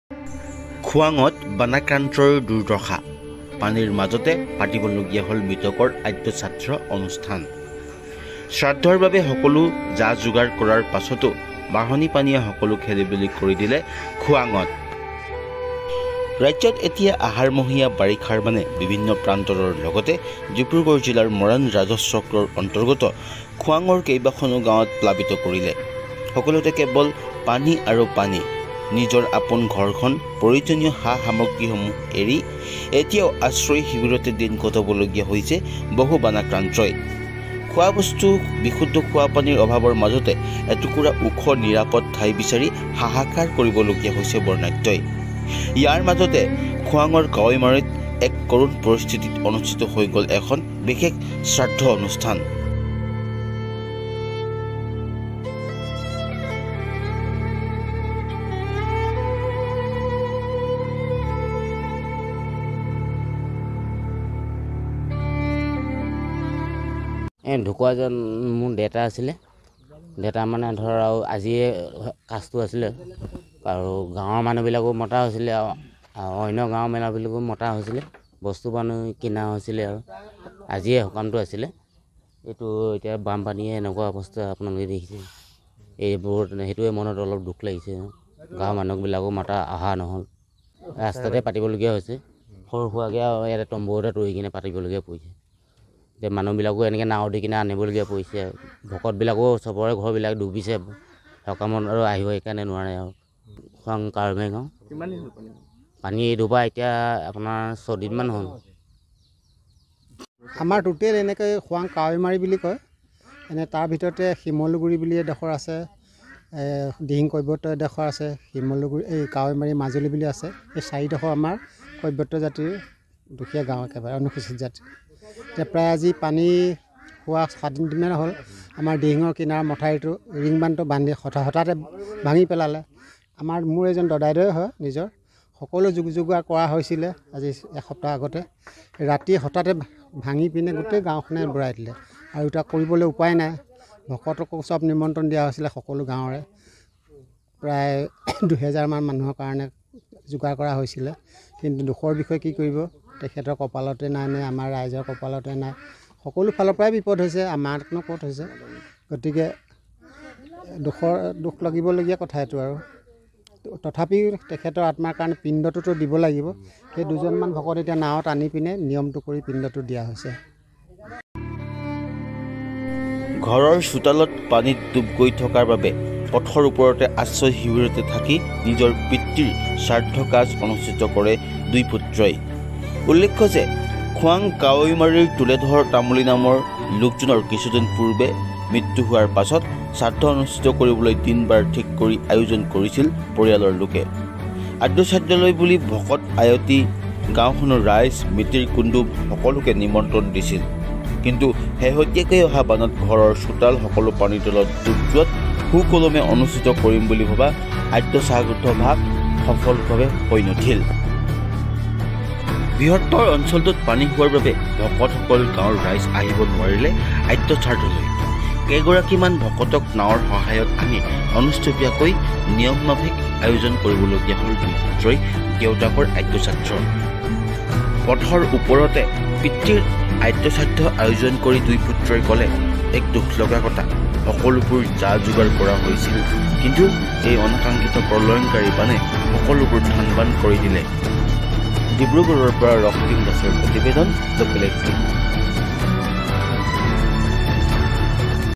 এক কৰুণ দৃশ্য,পথৰ ওপৰতে পিতৃৰ আদ্যশ্ৰাদ্ধ
নাৱেৰে আনিলে ভকত খোৱাঙত বানাক্ৰান্তৰ দুৰ্দশা,পানীৰ মাজতে পাতিবলগীয়া হ'ল মৃতকৰ আদ্যশ্ৰাদ্ধ অনুষ্ঠান।news18assamne